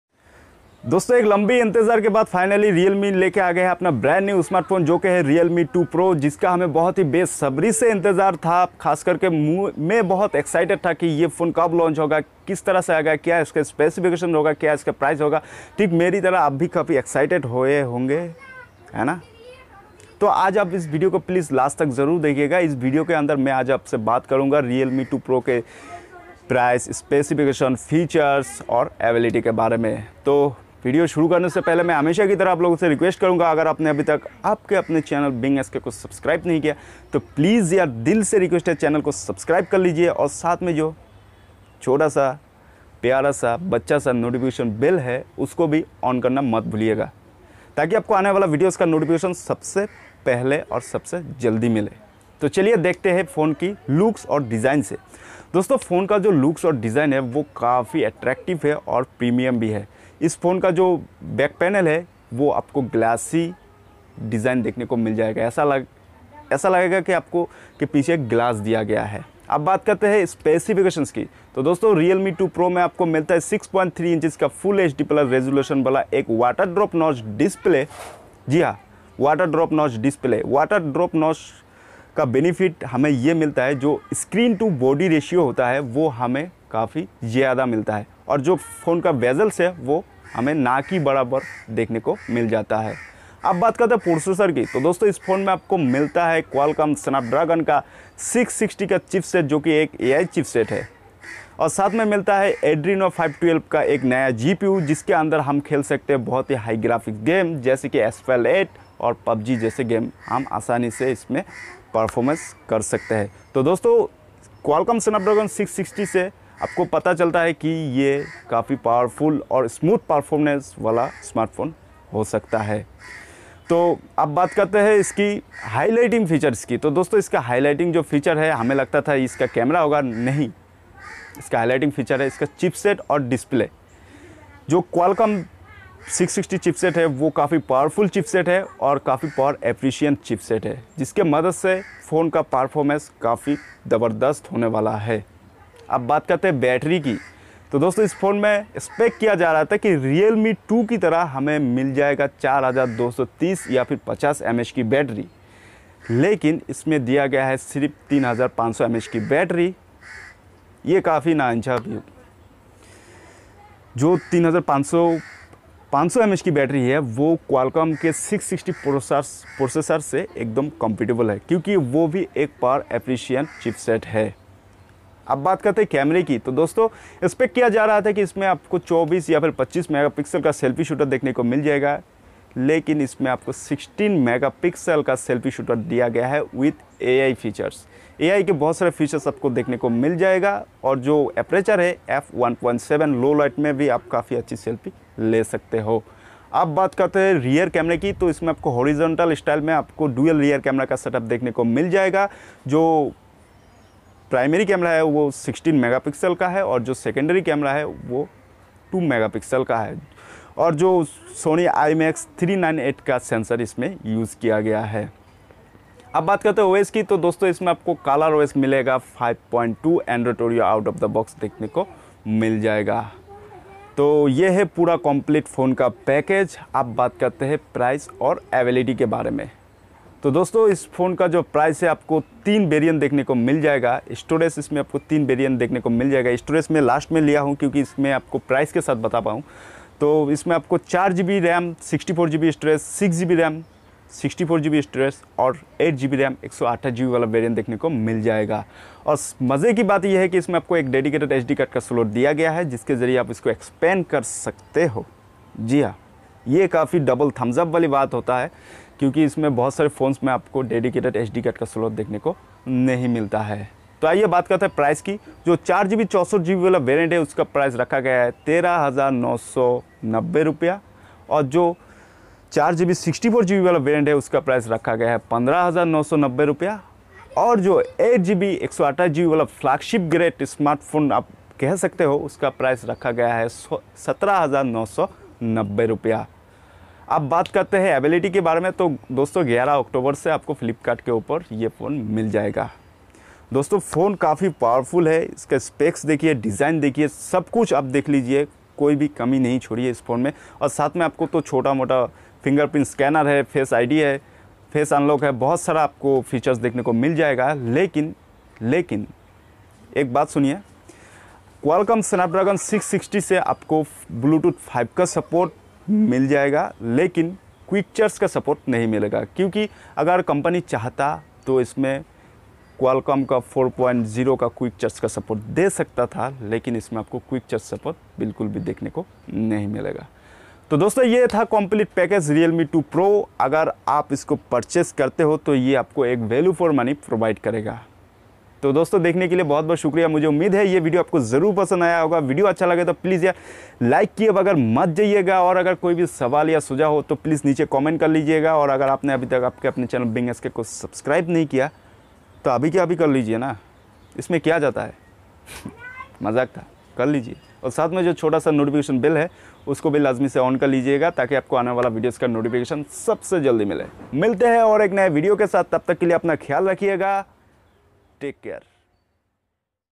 Realme 2 Pro Unboxing & Review in Hindi - Yeh Hai Asli Flagship Killer 🔥🔥🔥
Realme 2 Pro Unboxing, Review, First Look, Hands On, Over View, Features, Specification, Price in india

___Hello Friends main hoo aapke dost Arif Sk, aur aaj ke is video pe maine aapse Realme 2 Pro Smartphone ke bare main bataya hoo
​Thaks for Watching
                                     Being Sk
                                 beingsk
​Jay Hind
​Jay Bharat.